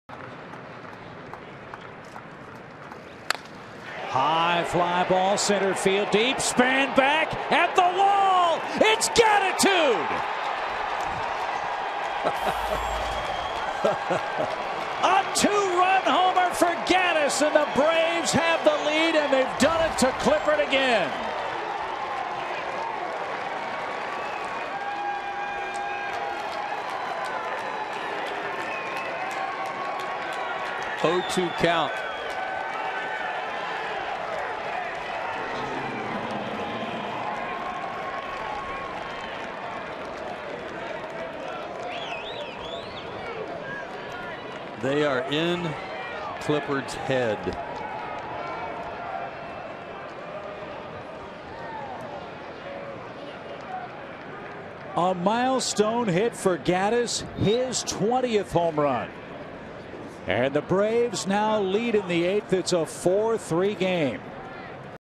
2013/09/17 Gattis' two-run dinger
ATL@WSH: Gattis' 20th homer gives Braves late lead